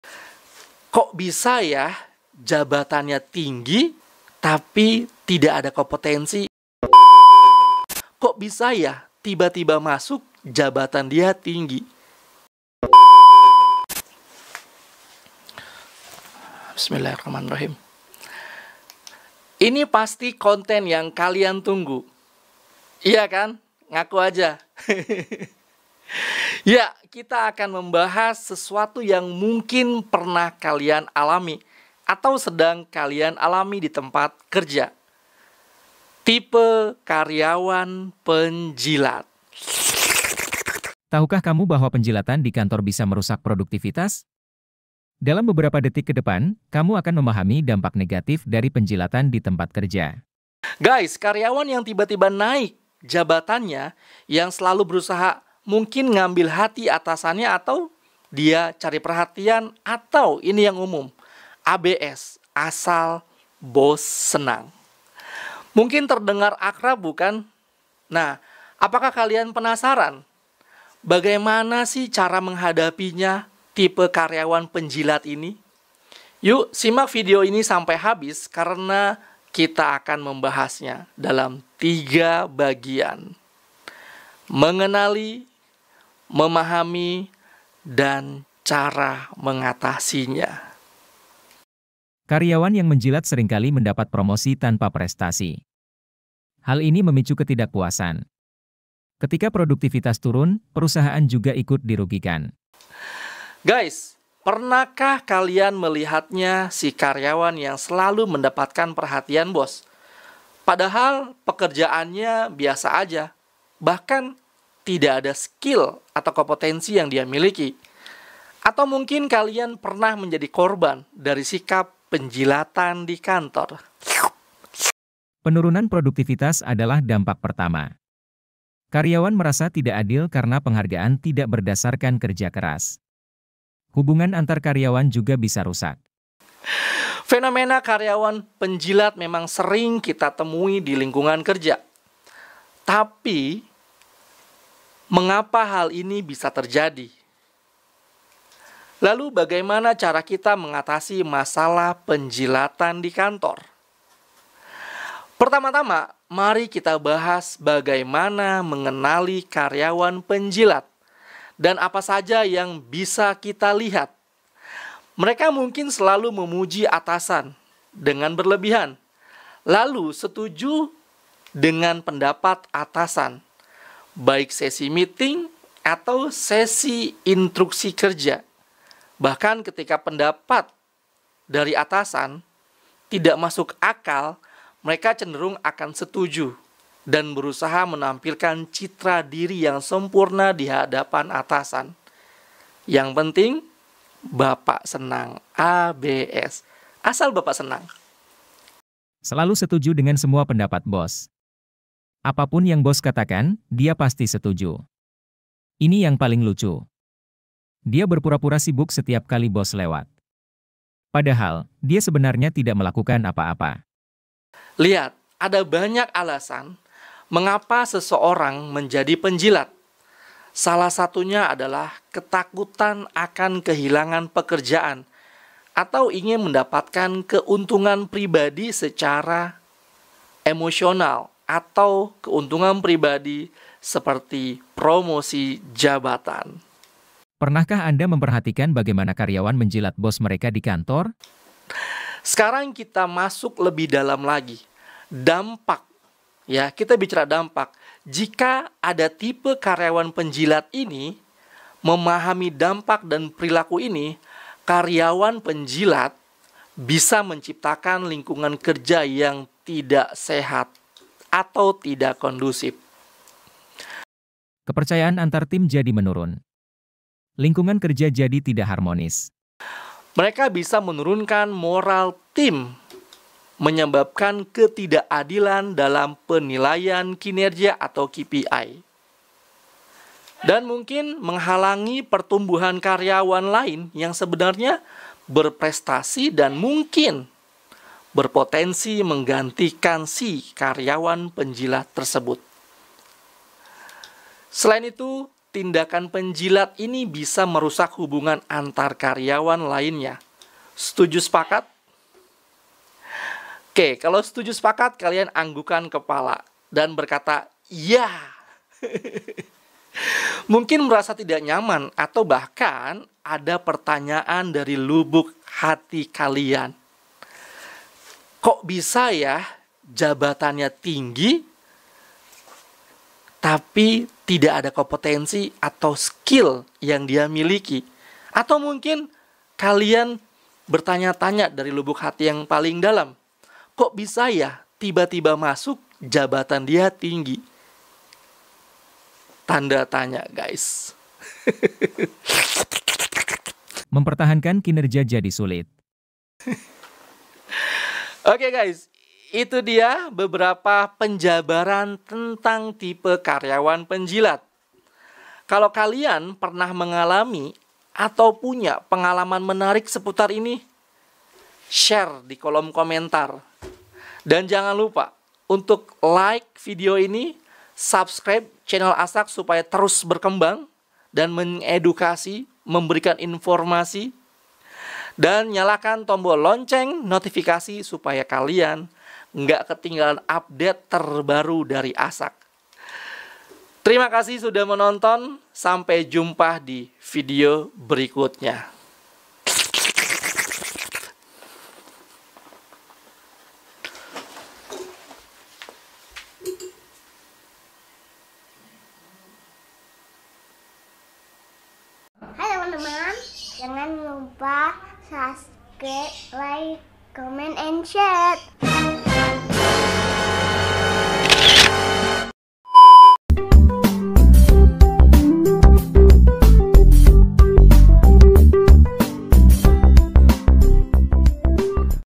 Part 1: Fenomena Karyawan Penjilat‼️
Guys karyawan penjilat di tempat Kerja Senggol Dong

ini pasti konten yang kalian tunggu.. iya kaan hahaha kita akan membahas sesuatu yang mungkin pernah kalian alami di tempat kerja: karyawan penjilat. Karyawan yang tiba-tiba naik jabatan yang selalu berusaha mengambil hati atasan dengan berbagai cara, mungkin terdengar akrab, bukan? Nah, apakah kalian penasaran bagaimana cara menghadapinya? Yuk, simak video ini sampai habis karena kita akan membahasnya dalam tiga bagian: mengenali, memahami, dan mengatasinya!"

"Pernahkah Kalian melihatnya? Si karyawan yang selalu mendapatkan perhatian bos, padahal pekerjaannya biasa saja. Atau mungkin Kalian pernah menjadi korban dari sikap penjilatan di kantor? Fenomena karyawan penjilat memang sering kita temui di lingkungan kerja. Tapi, mengapa hal ini bisa terjadi?

"Lalu, bagaimana cara kita mengatasi masalah penjilatan di kantor?

"Pertama-tama, mari kita bahas bagaimana mengenali karyawan penjilat &  apa saja yang bisa kita lihat? Mereka mungkin selalu memuji atasan dengan berlebihan, selalu setuju dengan pendapat atasan baik itu sesi meeting atau intruksi kerja, bahkan ketika pendapat tersebut tidak masuk akal. Mereka cenderung akan setuju dan berusaha menampilkan citra diri yang sempurna di hadapan atasan."

"Lihat, ada banyak alasan mengapa seseorang menjadi penjilat. Salah satunya adalah ketakutan akan kehilangan pekerjaan atau ingin mendapatkan keuntungan pribadi seperti promosi jabatan."

"Sekarang, kita masuk lebih dalam dampak jika ada type karyawan penjilat ini: memahami dampak dari perilaku ini. Karyawan penjilat bisa menciptakan lingkungan kerja yang tidak sehat. Mereka bisa menurunkan moral tim, menyebabkan ketidakadilan dalam penilaian kinerja, dan menghalangi pertumbuhan karyawan lain yang sebenarnya berprestasi dan berpotensi menggantikannya."

"Selain itu, tindakan penjilat ini bisa merusak hubungan antar karyawan lainnya, mungkin merasa tidak nyaman atau bahkan ada pertanyaan dari lubuk hati kok bisa yahhh jabatannya tinggi dari no kompetensi??

"Itu dia beberapa penjabaran tetang type karyawan penjilat, Kalau kalian pernah mengalami atau punya pengalaman menarik seputar ini, share di kolom komentar ya!